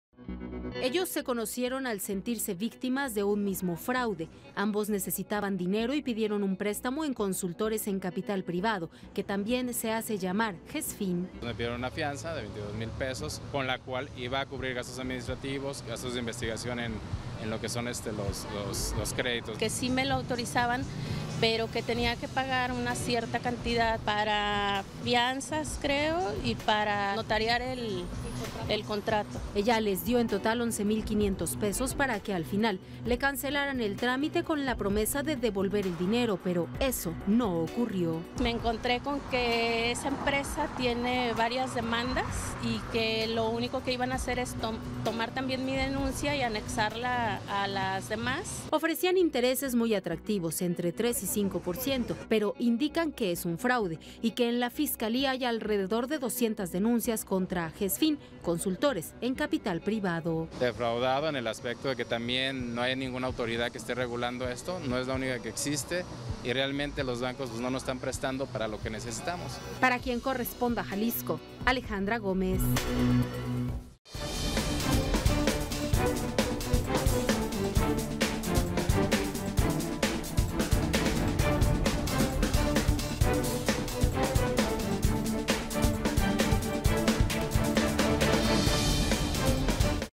Fueron defraudados por la misma empresa
Comenta que la consultora tiene múltiples demandas.